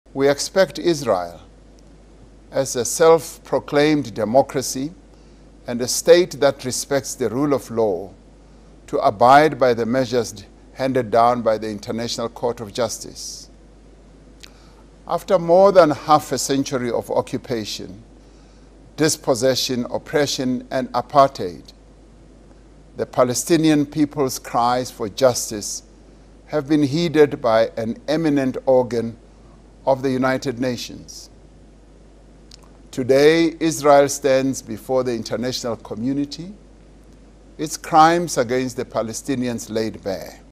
South Africa's President Cyril Ramaphosa reacts to the ICJ ruling
“Today Israel stands before the international community, its crimes against the Palestinian people laid bare."

South Africa's President Cyril Ramaphosa hails the International Court of Justice ruling against Israel as a step toward justice. The ICJ ruled earlier on Friday that Israel must commit to preventing a genocide, and has one month to prove it is doing so.